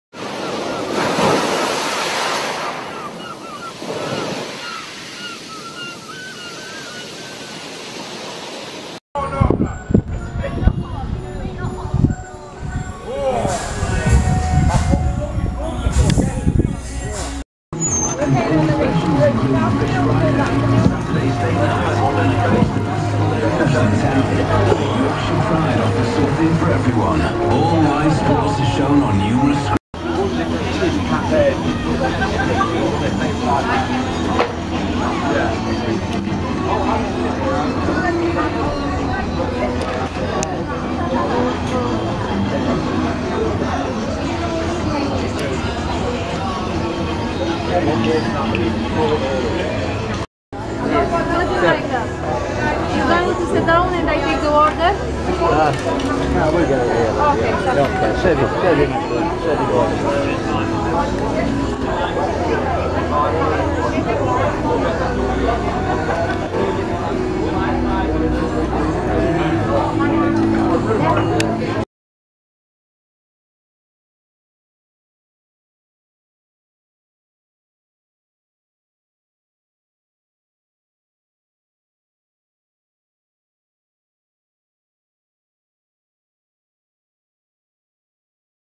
The Secret Fountain Benidorm - Benidorm Bars
The Secret Fountain bar Benidorm is located on the main strip. A relaxing atmosphere, cocktails and live entertainment.